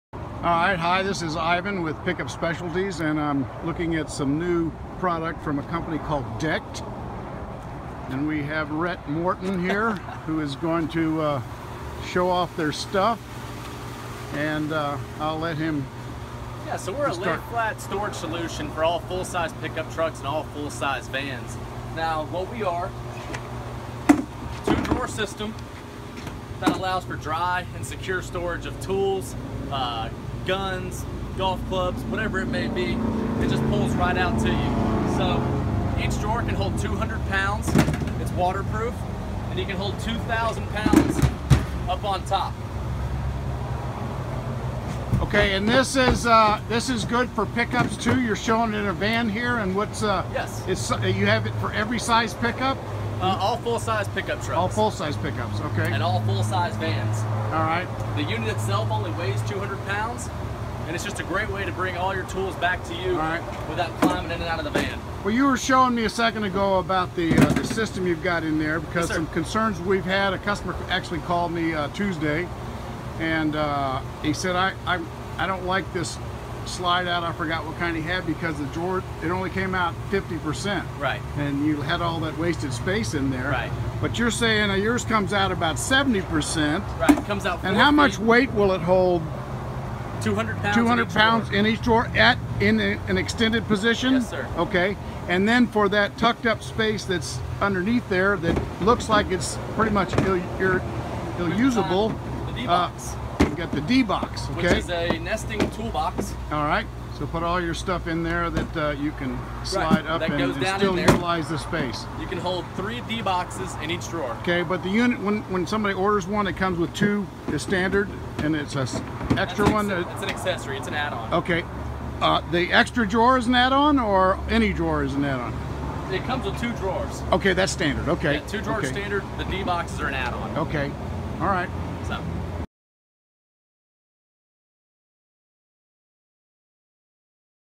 DECKED Van and Truck Bed Drawers and Cargo Organizers
See all of our Truck Accessories

DECKED revolutionizes truck bed storage and organization with an innovative, weatherproof and ergonomic solution for every full size pickup truck owner.  100% American made, the DECKED system features two secure full bed-length drawers that glide effortlessly on industrial-grade wheels beneath a 2,000lb payload-rated deck.  Constructed from rugged High Density Polyethylene (HDPE) and American steel, owners get ergonomic access to tools and equipment while maintaining full use of their pickup truck bed’s footprint.  Storage is customizable, and the system, once assembled, is easily installed and removed with no drilling or other alteration of the truck bed.  Forget crawling into your bed or trying to reach into your toolbox to retrieve your essentials, DECKED puts all of your critical gear right at your fingertips. Fits all full sized trucks made since the late 90’s with 5'6"-6'6" beds.